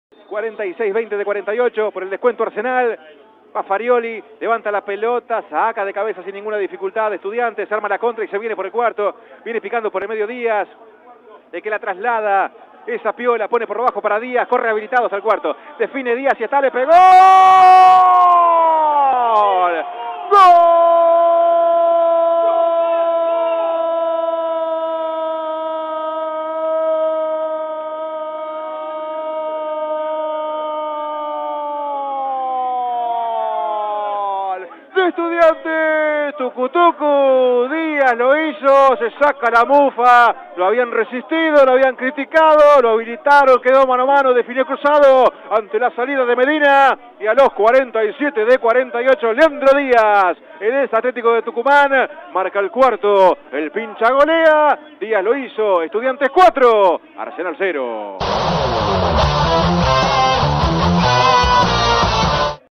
EDLP | Gol de Leandro Diaz vs Arsenal
El gol de Leandro Diaz (Estudiantes), frente a Arsenal, en la voz de Marcelo Gisande por Keops FM 90.1 LaPlata + Pinar Beach FM 106.7